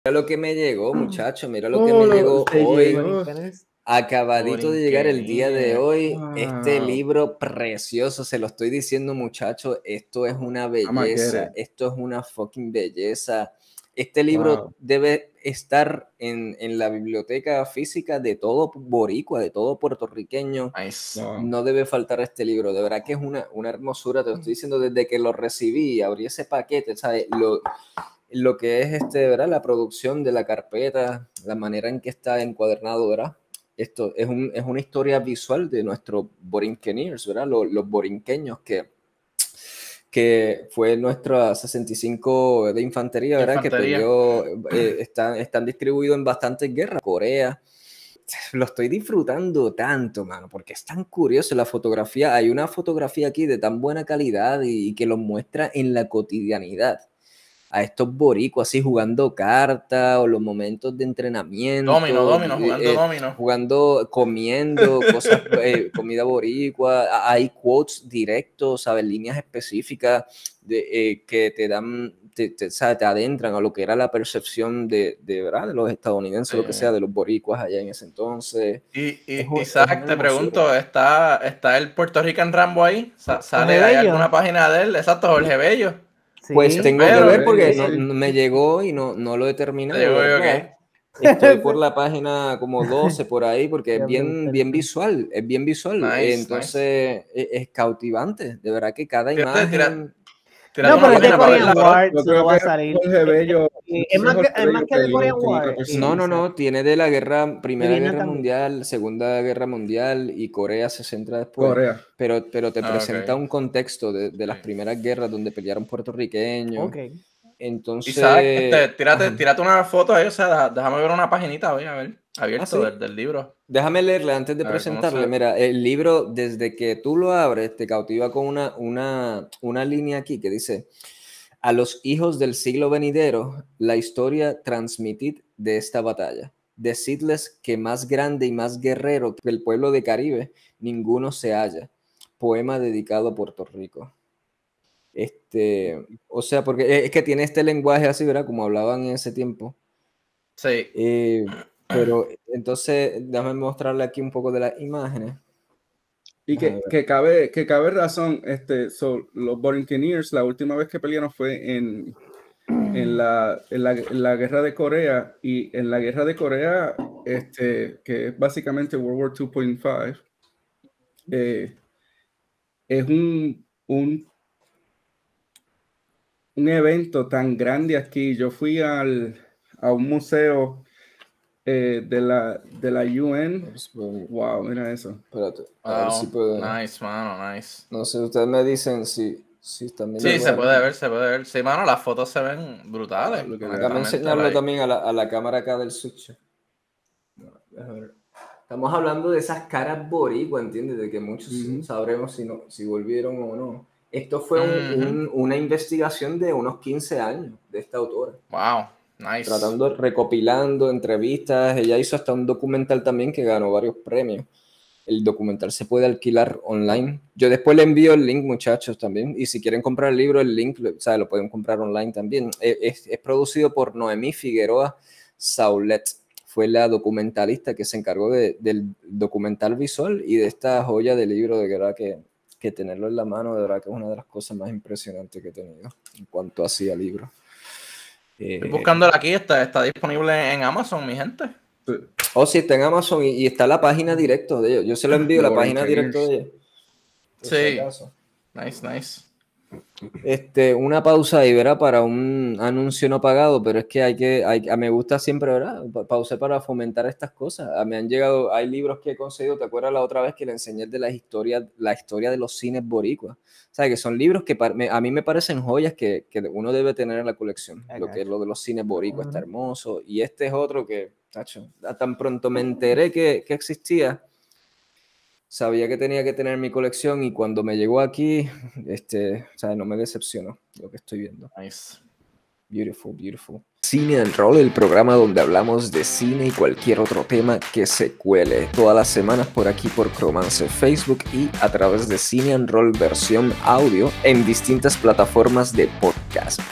The Borinqueneers: La historia del 65 de infanteria
Nos llegó el espectacular libro de Noemí Figueroa Soulet, que presenta visualmente la historia de los Borinqueneers.